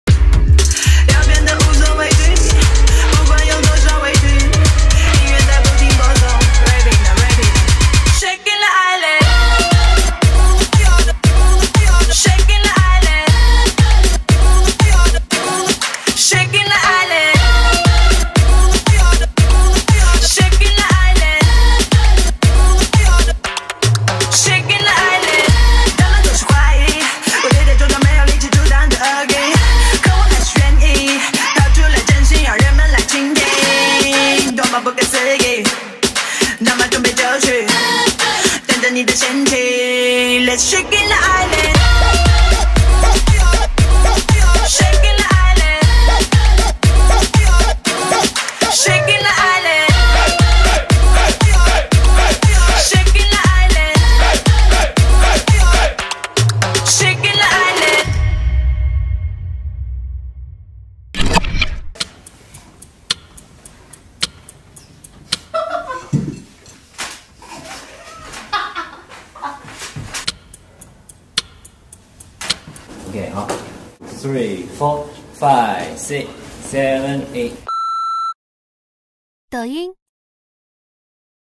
【李斯丹妮|Danny Lee】抖音Tiktok李斯丹妮白衬衫舞蹈20201104
@李斯丹妮：“高能星打卡 李斯丹妮白衬衫舞蹈 新鲜的🥚 新鲜的dance”